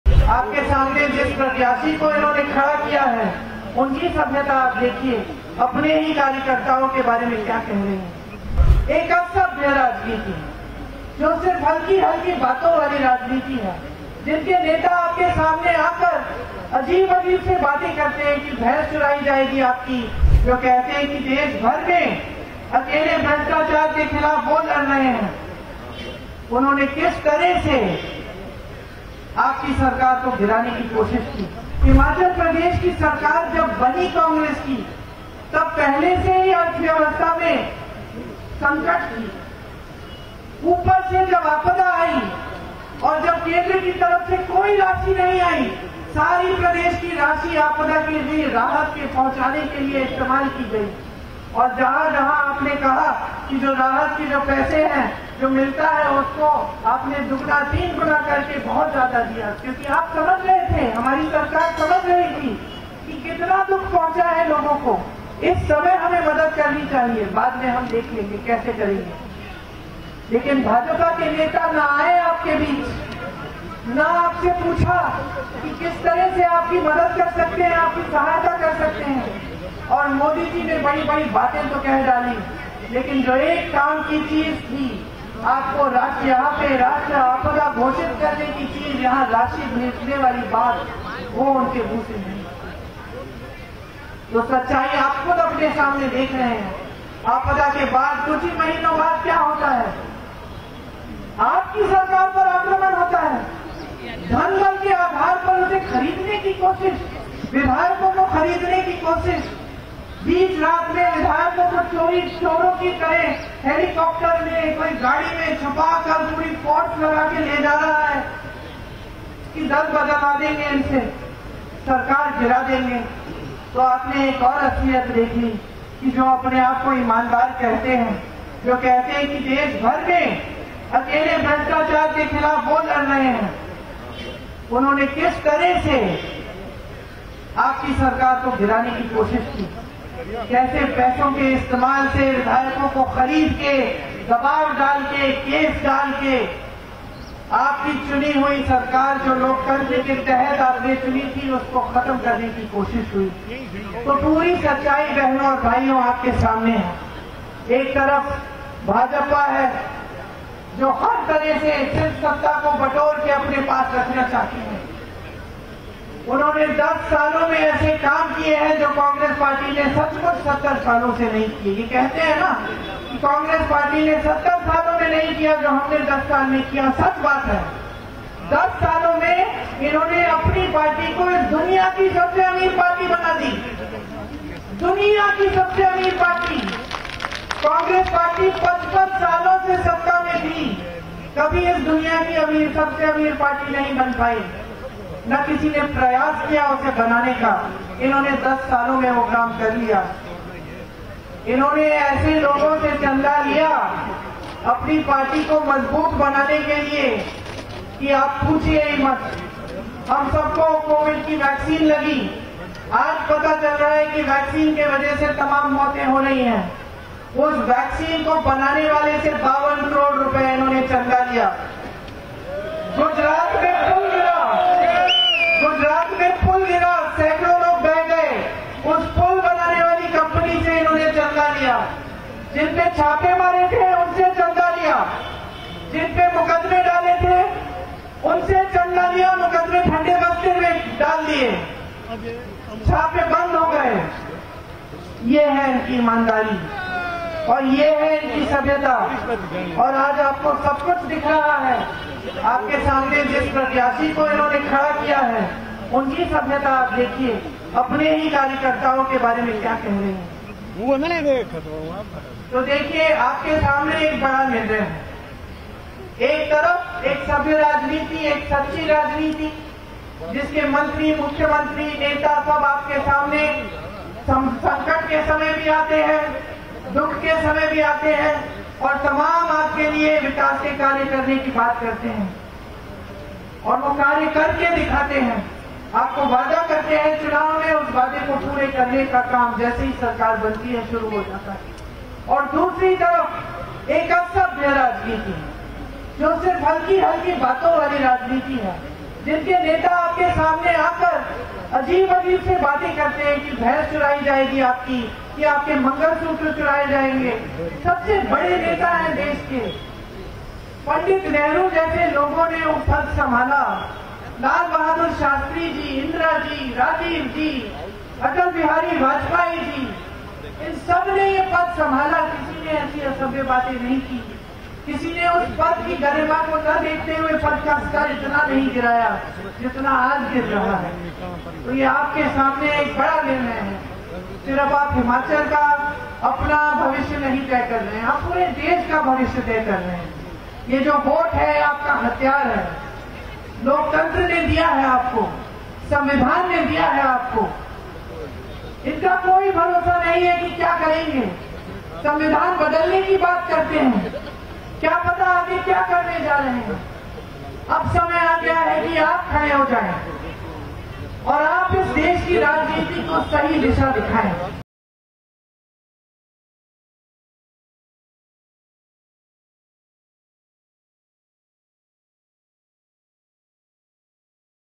"उनकी सभ्यता देखिये..." Priyanka Gandhi का Kangana Ranaut पर बड़ा हमला | Lok Sabha Election 2024
कांग्रेस (Congress) महासचिव प्रियंका गाँधी वाड्रा (Priyanka Gandhi Vadra) ने कुल्लू (Kullu) में जनसभा (Public Rally) के दौरान बीजेपी (BJP) पर जोरदार हमला किया. उन्होंने मंडी लोकसभा सीट (Mandi Constituency) से बीजेपी उम्मीदवार कंगना रनौत को उनके बयानों को लेकर आड़े हाथों लिया. कंगना रनौत (Kangana Ranaut) के बयानों पर तंज कसते हुए प्रियंका गाँधी ने कहा कि, "आज आपको सब कुछ दिख रहा है. आपके सामने जिस प्रत्याशी को इन्होने खड़ा किया है उनकी सभ्यता आप देखिये...अपने ही कार्यकर्ताओं के बारे में क्या कह रही हैं..."